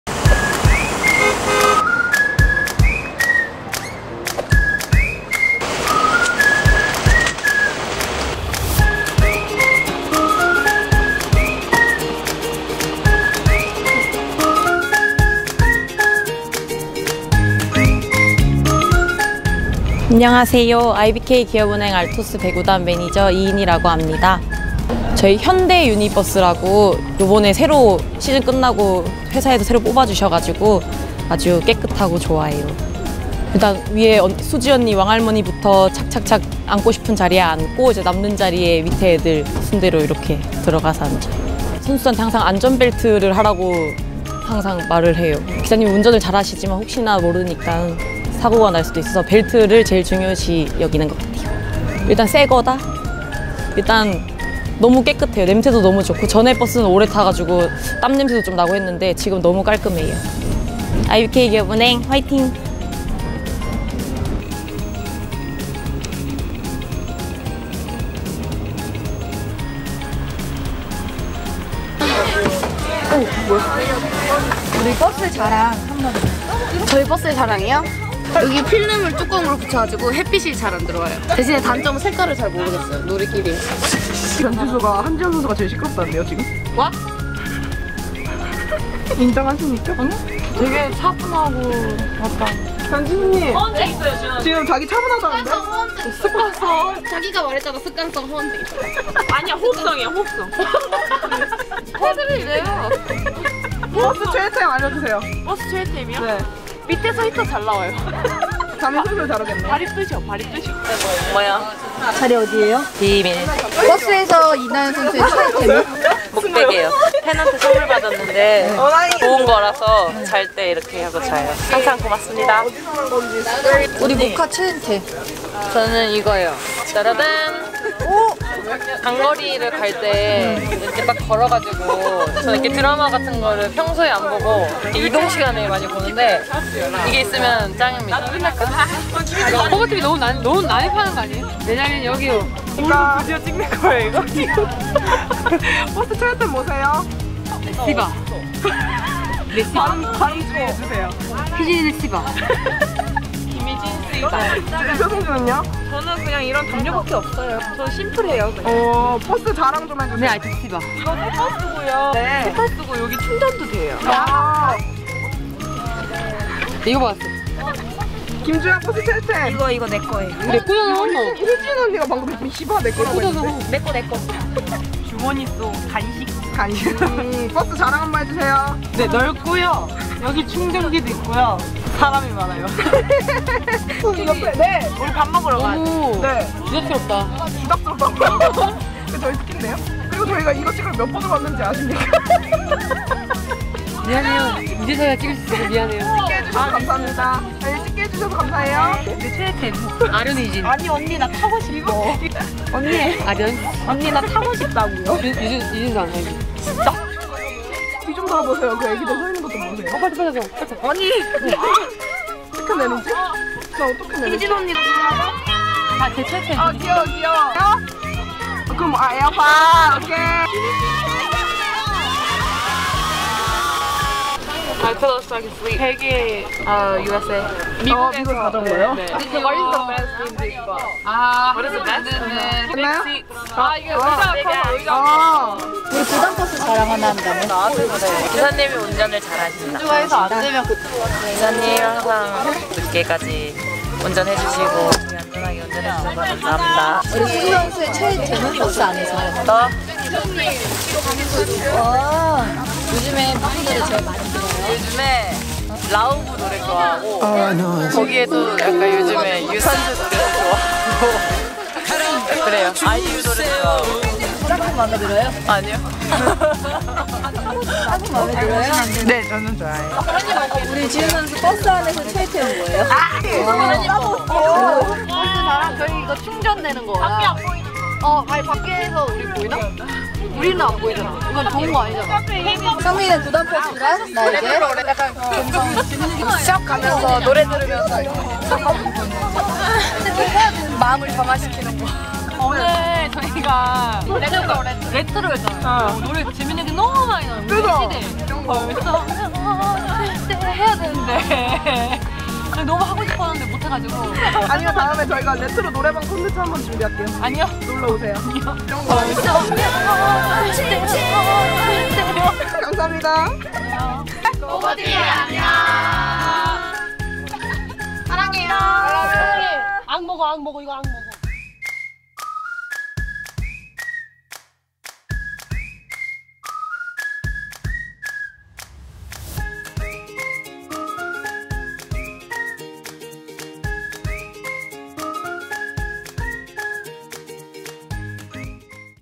[구단버스대탐방] '드디어 탔다!!!' IBK기업은행 구단버스★대공개★
코보티비 대기획 🚐구단버스 대탐방🚐
열두번째 구단버스는 IBK기업은행의 구단버스!
기업이들이 버스에서 가장 좋아하는 아이템은 ㅅㅂ???

(OK저축은행은 구단 자체컨텐츠로 버스 촬영을 진행하오니 참고부탁드립니다.)